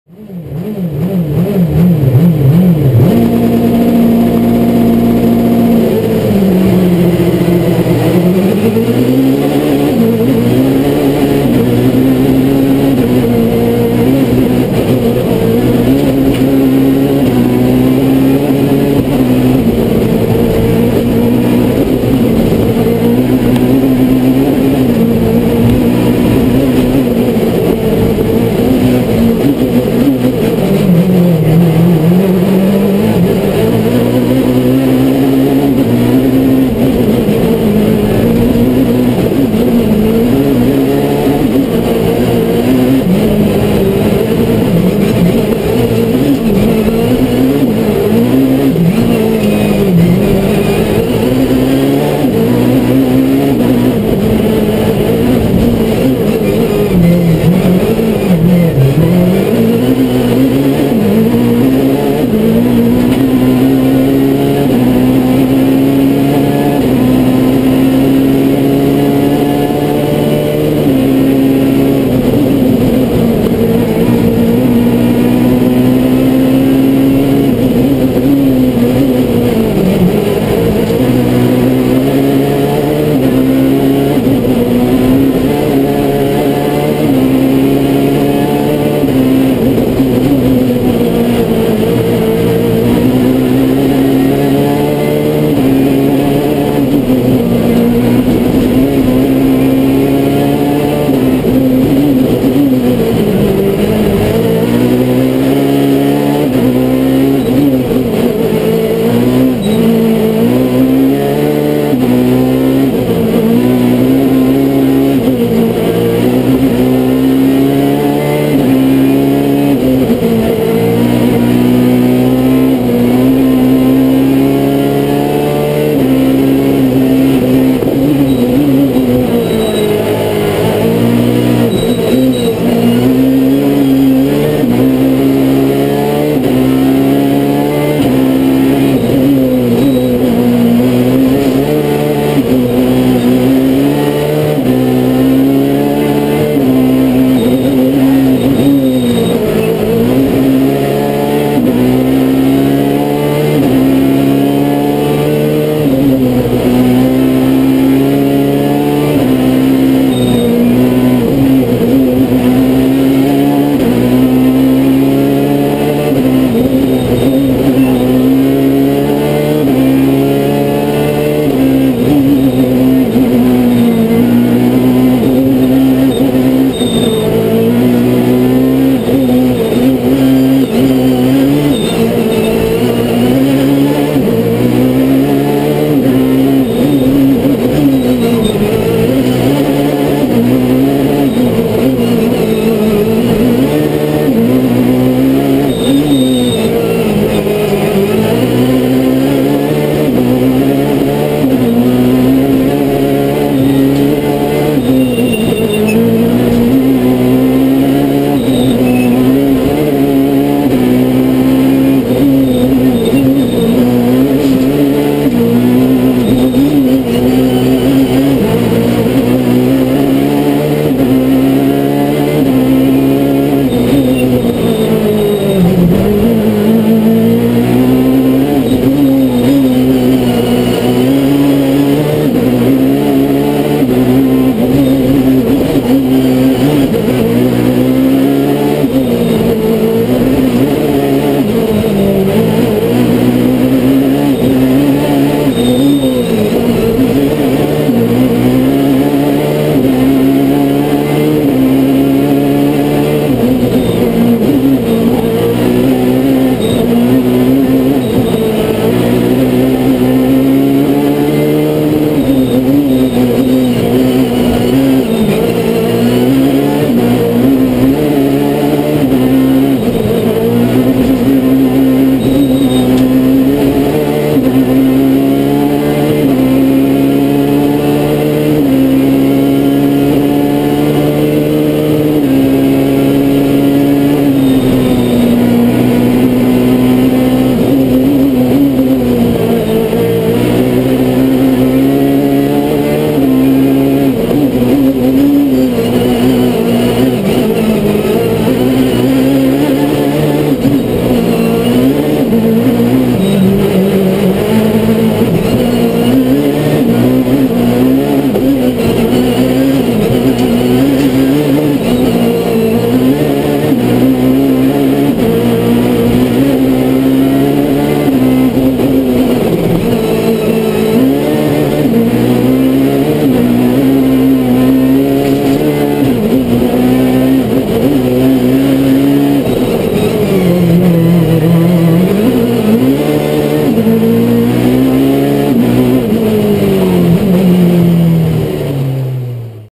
Onboard Dani Sordo Citroen Xsara Kit Car Rallye Isla de Tenerife 2013 TC-5
Onboard de Dani Sordo y Pablo Marcos con el Citroen Xsara de Auto Laca en el Rallye Isla de Tenerife 2013 en el tramo 5 de Arico